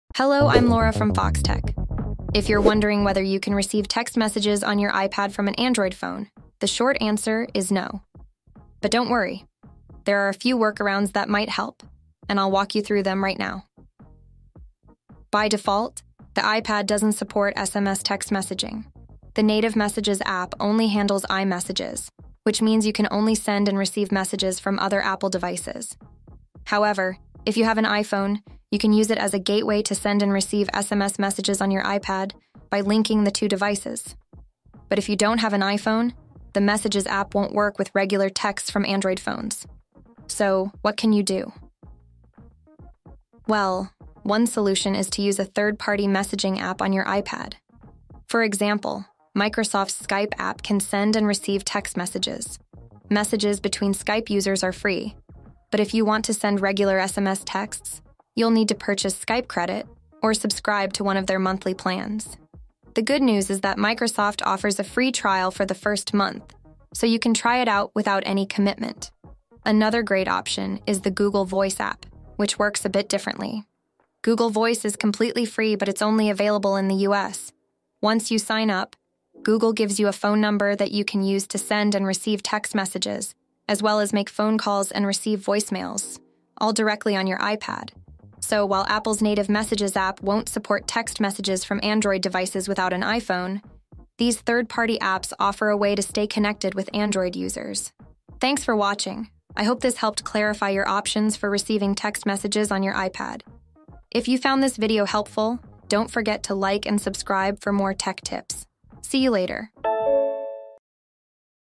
Can I Receive Text Messages on my iPad from an Android Phone (explained)
In this video, I’ll explain whether you can receive text messages on your iPad from an Android phone.